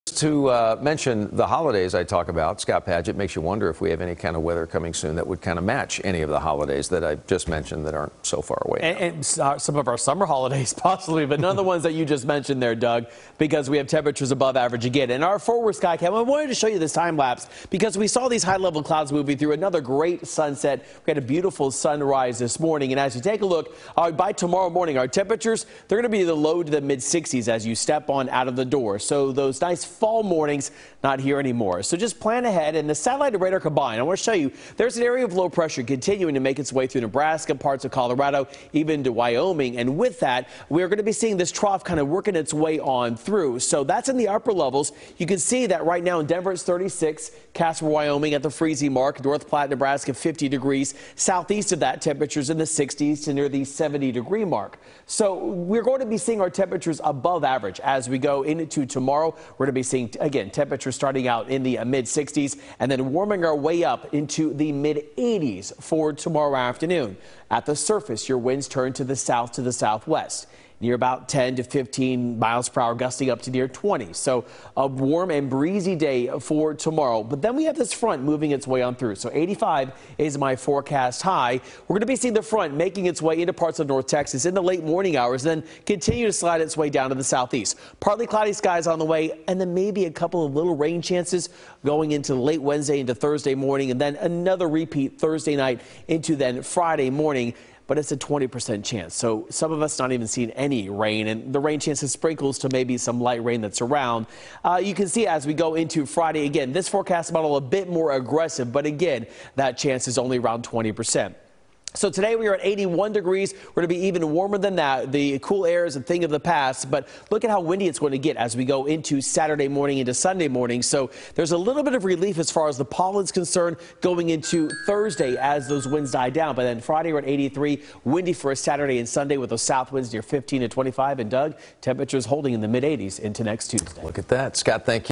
Warm And Breezy Wednesday
The high will be in the low 80s.